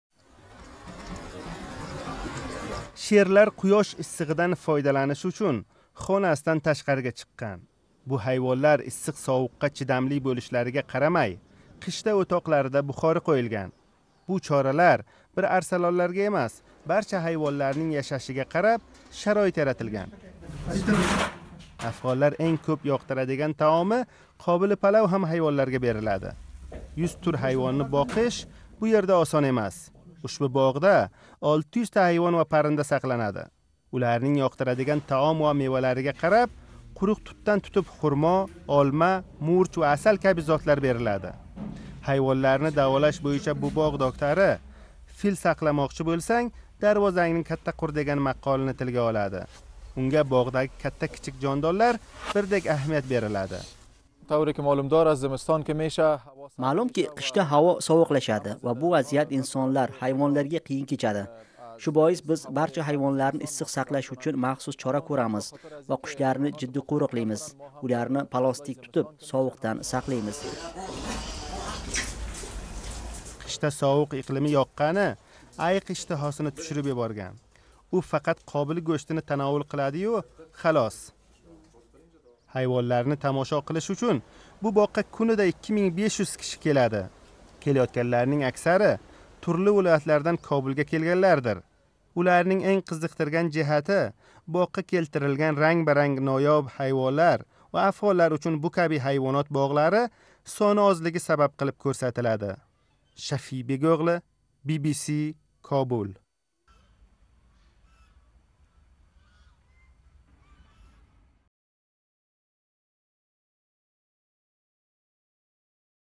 Паловхўр маймунлар - Кобул зоопаркига саёҳат - BBC Uzbek
Афғонистон қишлари совуқлигини ўз даврида Кобулни пойтахт қилган шоҳ ва шоир Заҳириддин Муҳаммад Бобур "Бобурнома"да кўп ва хўб тасвирлаган.

Афғонистондаги қушлар ва бошқа турли-туман ҳайвонот ва наботот дунёси ҳам Бобурнинг назаридан четда қолган эмас.

Аммо шу кунлар Афғонистон пойтахтининг якка-ю ягона ҳайвонот боғига сафар қилган Би-би-си мухбирлари "Бобурнома"да учрамайдиган ажойиб манзарага дуч келганлар: Ўзбек паловини паққос тушираётган маймунлар ва айиқ полвон!

Сиз ҳам томоша қилинг!